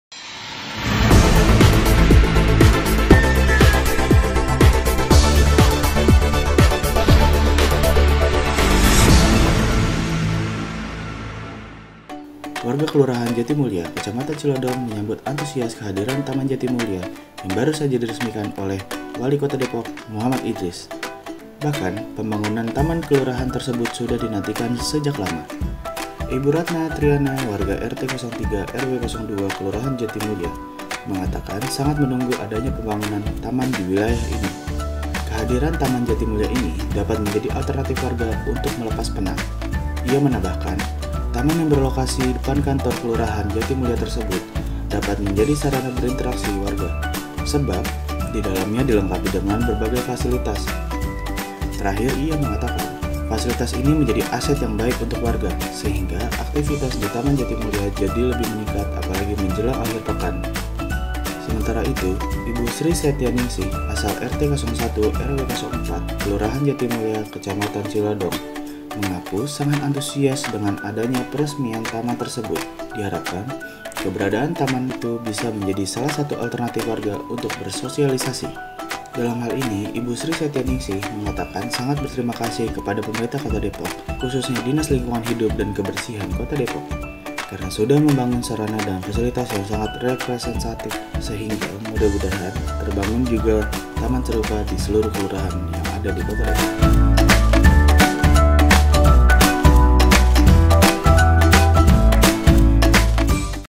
Kehadiran Taman Jatimulya Disambut Antusias Warga
berita.depok.go.id

Warga Kelurahan Jatimulya, Kecamatan Cilodong menyambut antusias kehadiran Taman Jatimulya yang baru saja diresmikan oleh Wali Kota Depok, Mohammad Idris. Bahkan pembangunan taman kelurahan tersebut sudah dinantikan sejak lama.

“Kami sangat menunggu adanya pembangunan taman di wilayah ini. Allhamdulillah saat ini terealisasi. Kehadiran Taman Jatimulya bisa menjadi alternatif warga untuk melepas penat,” ujar Ratna Triana, warga RT 03, RW 02, Kelurahan Jatimulya usai mengikuti kegiatan peresmian taman tersebut, Sabtu (08/02/20).

Menurutnya, taman yang berlokasi di depan Kantor Kelurahan Jatimulya tersebut, dapat menjadi sarana berinteraksi warga. Sebab, di dalamnya dilengkapi dengan berbagai fasilitas yaitu lapangan futsal, taman bermain anak, sarana refleksi untuk lansia, selfie ornamen, teater terbuka serta fasilitas lainnya yang bisa digunakan untuk umum.

“Fasilitas ini menjadi aset yang baik untuk warga. Sekarang aktivitas di Taman Jatimulya jadi lebih meningkat apalagi menjelang akhir pekan. Kami berjanji akan mengelola dan memelihara taman ini dengan baik,” ucap wanita yang juga menjabat sebagai Ketua Pokja I PKK Kelurahan Jatimulya ini.

Sementara itu, warga lainnya, Sri Setya Ningsih asal RT 01, RW 04, Kelurahan Jatimulya, Kecamatan Cilodong mengaku sangat antusias dengan adanya peresmian taman tersebut. Diharapkan, keberadaan taman itu bisa menjadi salah satu alternatif warga untuk bersosialisasi.

“Kami sangat berterimakasih kepada Pemerintah Kota (Pemkot) Depok khususnya Dinas Lingkungan Hidup dan Kebersihan (DLHK) Kota Depok, karena sudah membangun sarana dan fasilitas yang sangat representatif. Mudah-mudahan terbangun juga taman serupa di seluruh kelurahan yang ada di Kota Depok,” ungkapnya. (JD 08/ED 01/EUD 02)
Editor Video: Rahman & Kevin
Videografer: Okky Mardiansyah
Penanggung Jawab Video : Yulia Sohim

Diskominfo Kota Depok